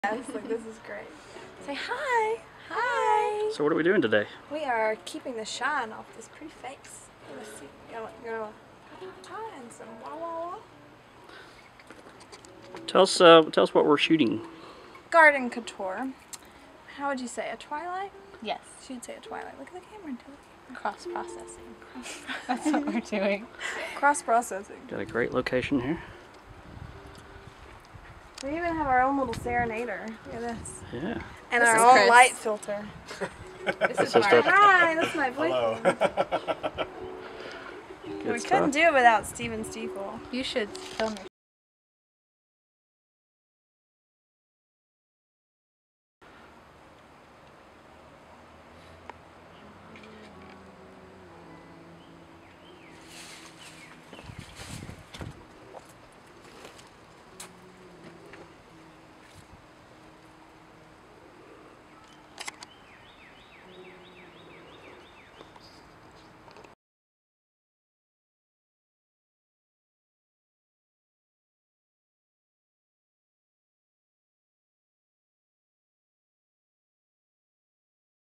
Garden Couture Photo Shoot by Steven Stiefel
Behind the scenes of a fashion photo shoot by Chattanooga TN photographer Steven Stiefel with models Tara Lynn Overby and Emily Caroline in Atlanta, Georgia.